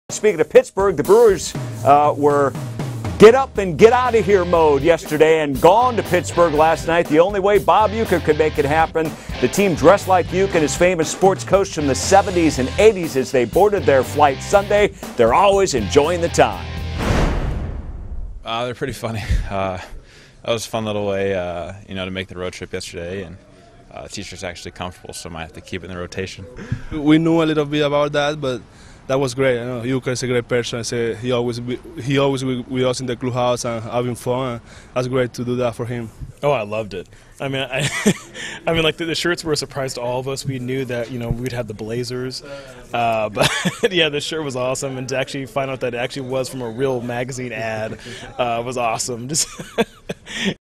Brewers take "Uecker trip" to Pittsburgh
Milwaukee traveled to Pittsburgh in style in honor of their Hall of Fame radio broadcaster Bob Uecker.